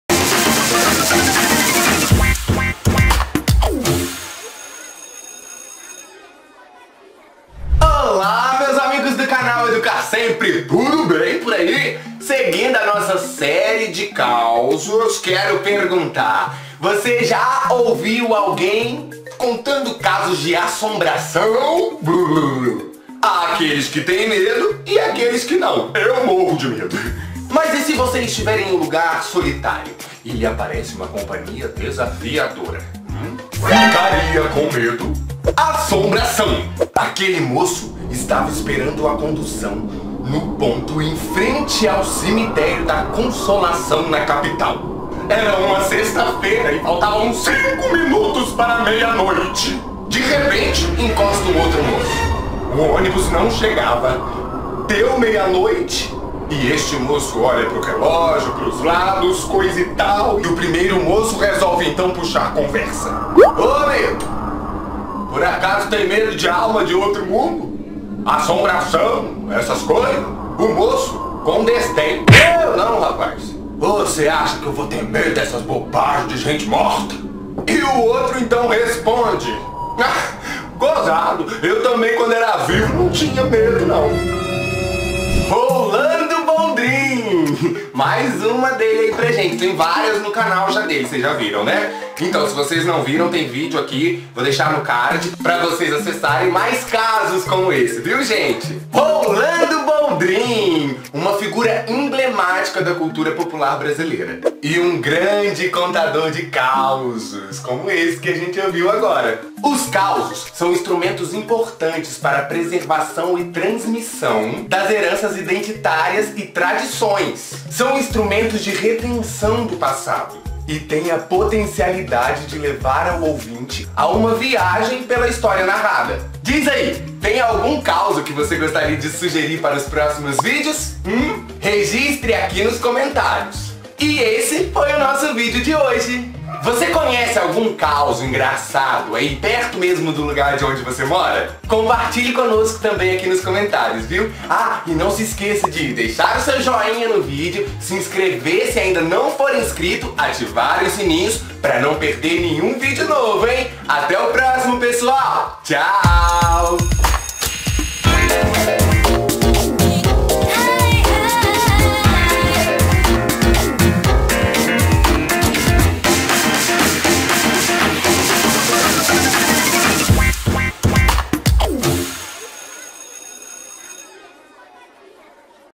Causos: Assombração
Mais um causo de Rolando Boldrin, que tal um pouco de humor junto com cultura?